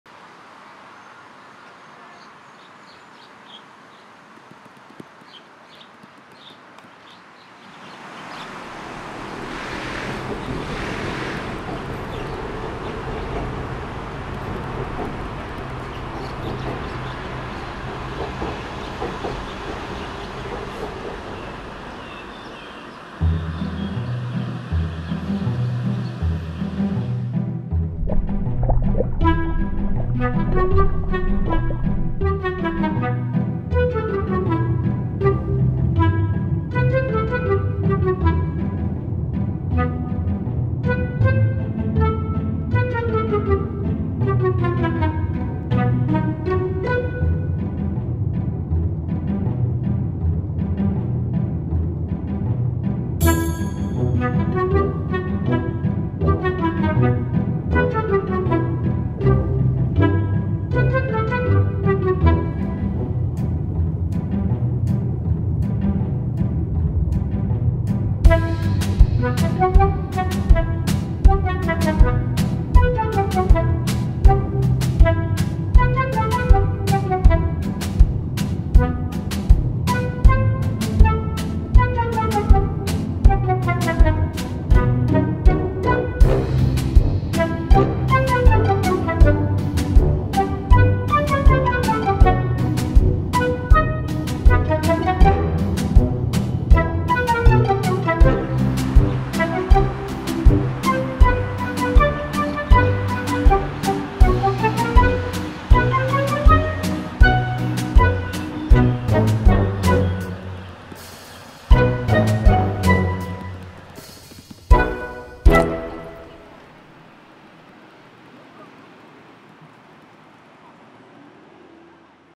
Small world
Another hot sunny day at Jatujak park. There's life flourishing in a medium size lotus jar, by using macro cinematography technique you'll see them in another perspective. Enjoy!

Filmed with canon C100 mark ii and Astrhori 28mm F13